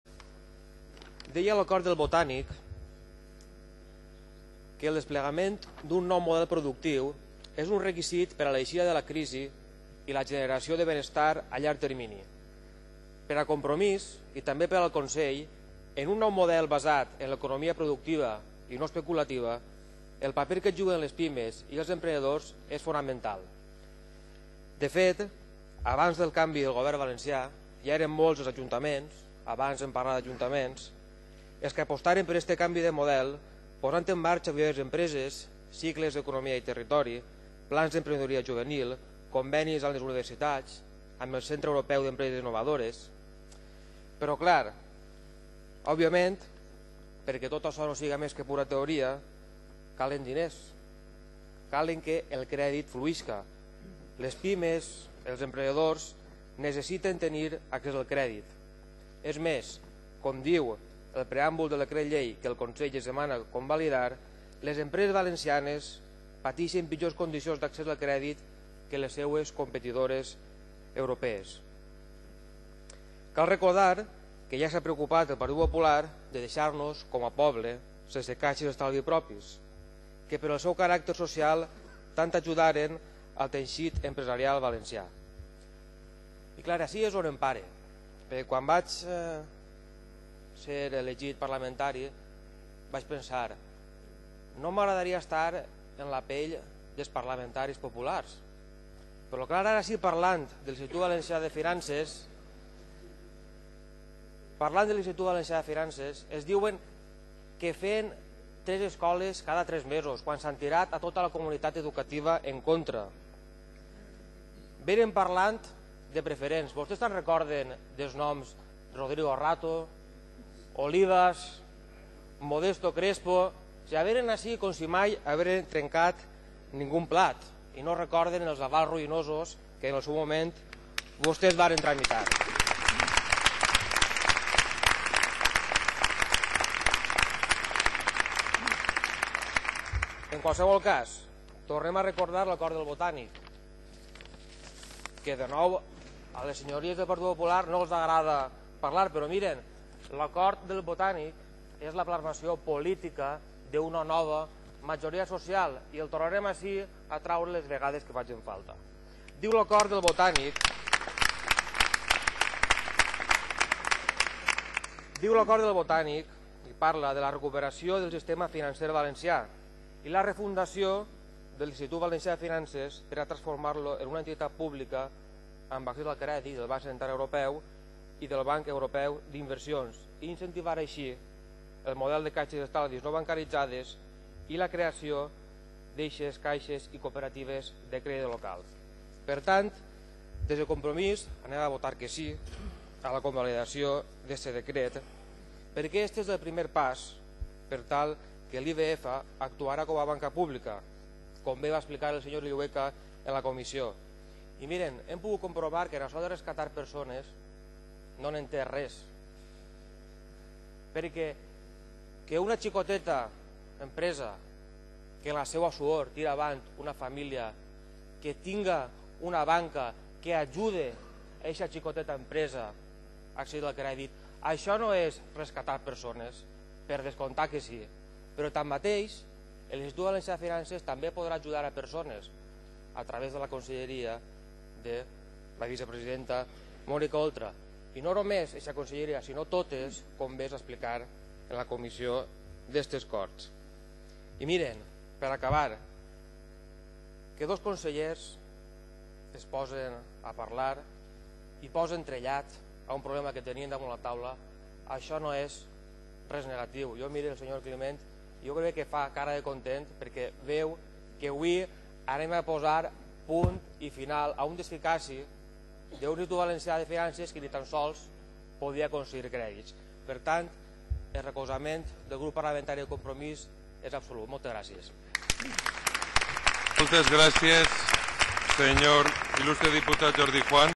Jordi Juan: "Per fi s'acaba amb el desficaci de què l'IVF no puguera donar crèdits"
El diputat Jordi Juan (Compromís) mostra el suport del seu grup a la modificació legislativa que permetrà concedir crèdit a l'Institut Valencià de Finances, el primer pas per a tindre una banca pública pròpia.